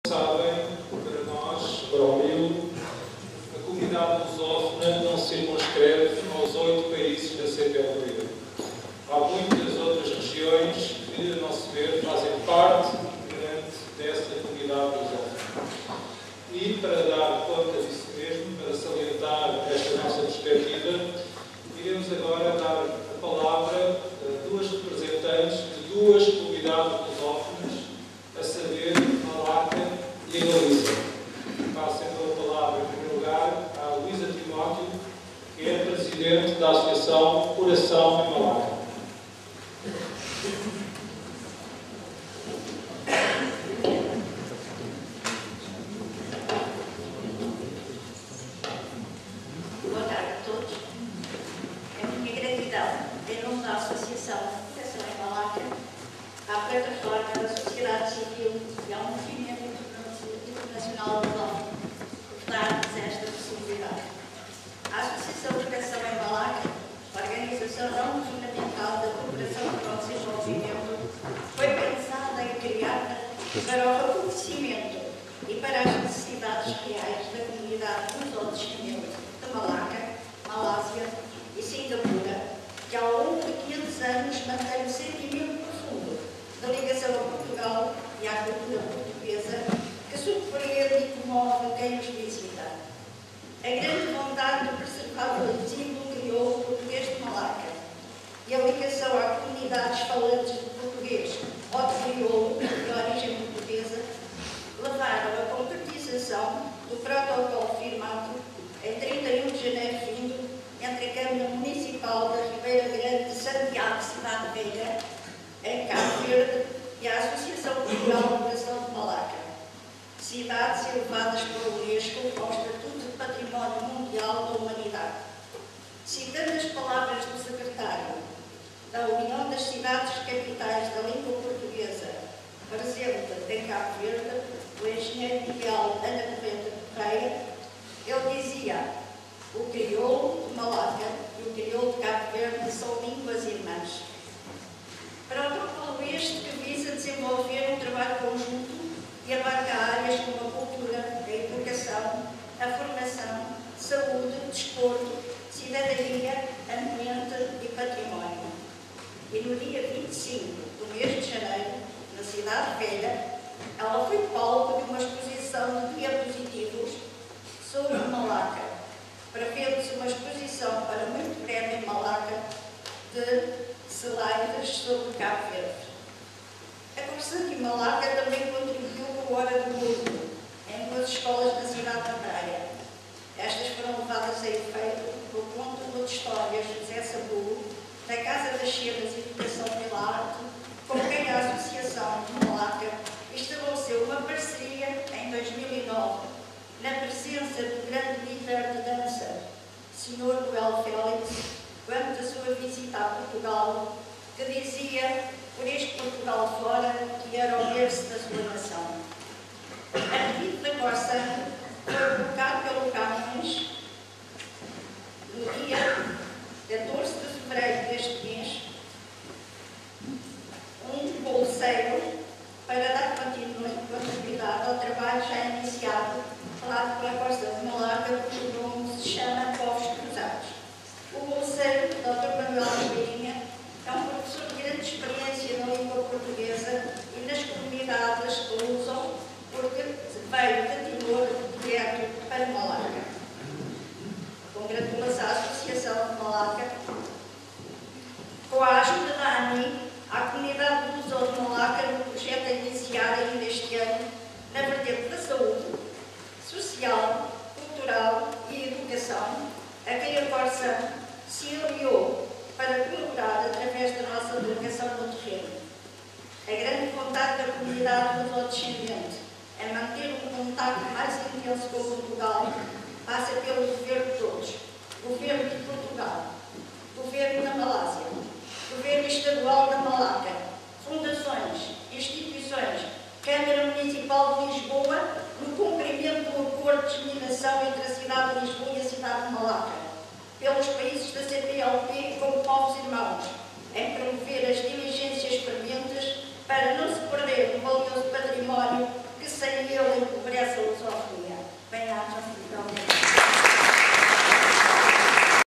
X Fórum PASC: Encontro Público "A Importância da Lusofonia" (10)
Luísa Timóteo (Associação Cultural Coração em Malaca)